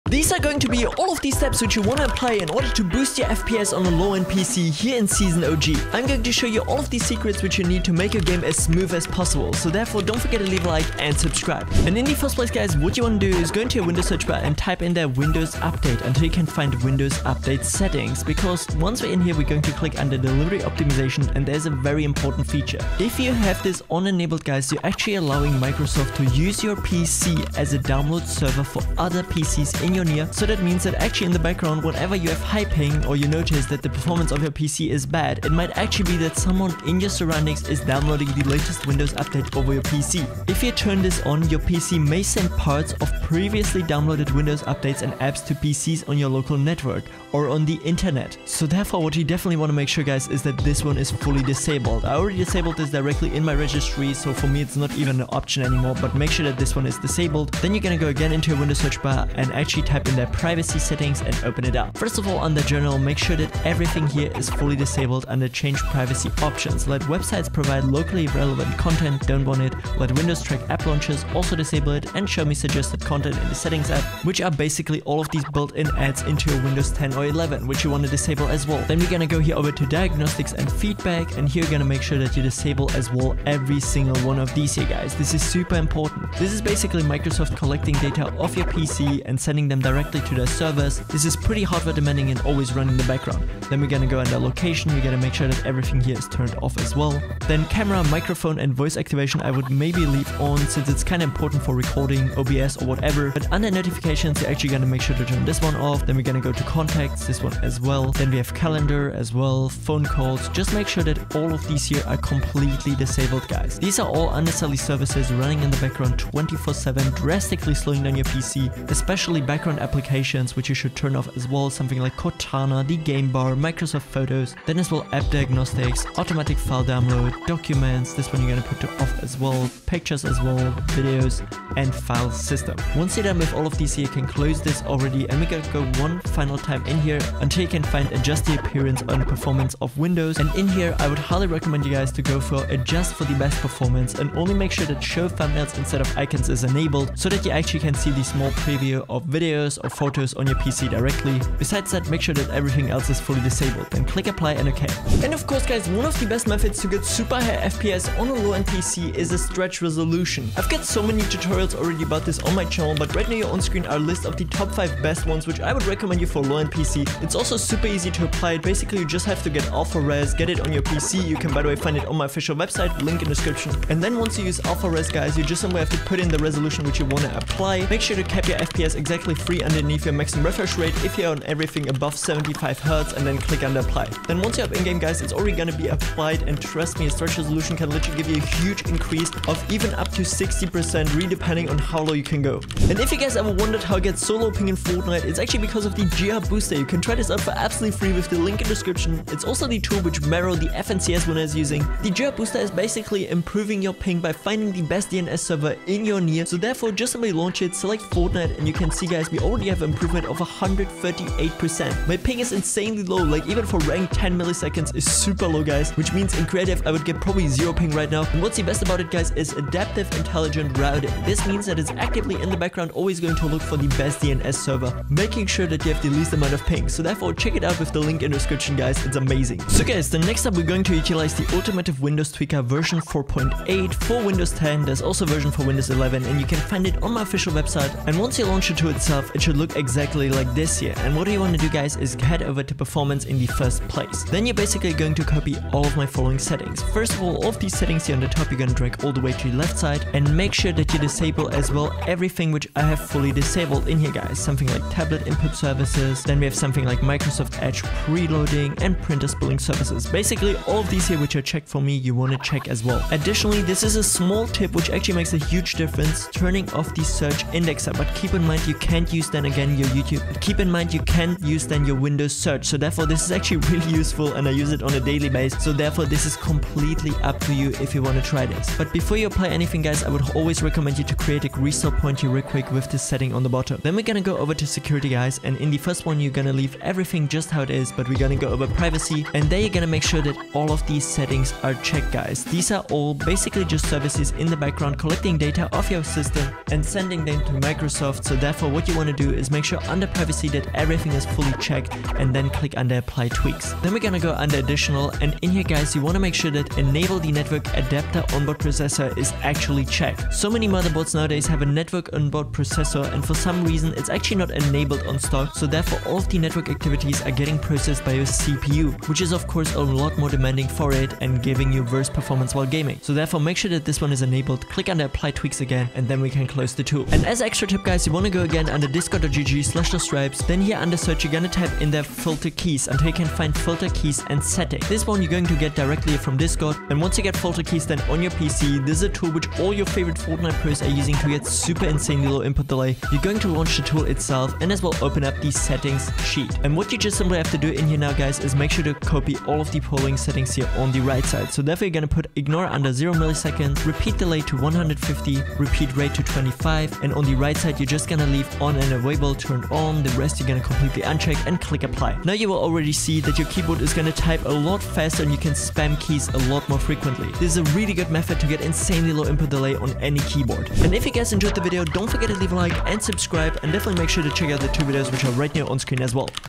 Best Fortnite Fps Boost LOW END PC! ✅ (Max FPS & 0 Delay)
If you're looking for a Fortnite fps boost that will let you play at a high frame rate on a low end PC, then look no further! In this video, I'll show you how to get 0 delay and 0 ping in Fortnite, so you can play at the highest possible frame rate with low input delay.

This best Fortnite fps boost low end PC tutorial is perfect for players on a budget who want to improve their game without spending a lot of money. By following my guide, you'll be able to play Fortnite at a high frame rate on a low end PC!

IMPORTANT (0:00)
Best Windows Settings (0:11)
Stretched Res (2:35)
Pro Tool (3:12)
Tweak Windows (3:49)
0 Delay Tool (5:27)

► Title: Best Fortnite Fps Boost LOW END PC! ✅ (Max FPS & 0 Delay)